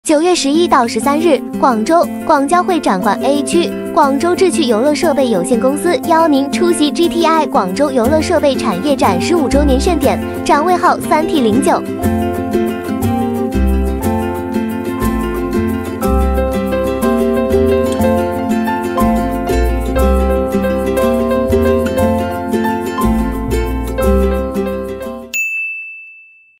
Zhiqu Meet you at the 15th GTI Asia China Expo on September 11-13
9月11-13日，广州广交会展馆A区，志趣游乐 邀您出席 第15届GTI广州游乐展，展位号：3T09
Zhiqu Meet you at the 15th GTI Asia China Expo at Area A, China Import and Export Fair Pazhou Complex, Guangzhou on September 11-13. Booth NO. : 3T09